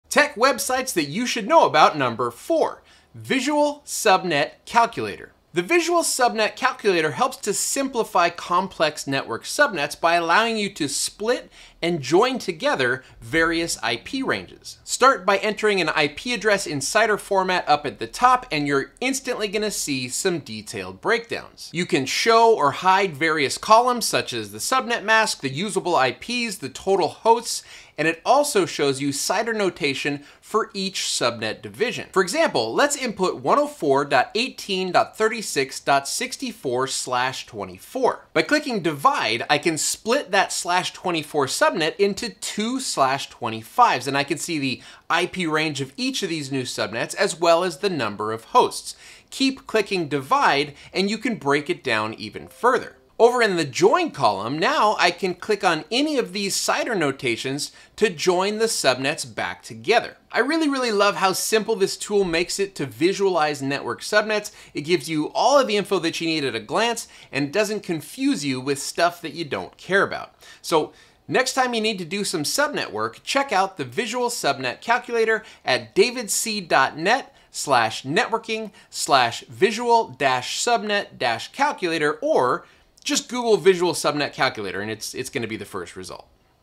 Visual Subnet Calculator is a powerful free tool - try it today!
Visual Subnet Calculator is an awesome tool that takes the guesswork out of working with network subnets.

Amazon Wish List

Crosstalk Solutions offers best practice phone systems and network/wireless infrastructure design/deployment.  for more info!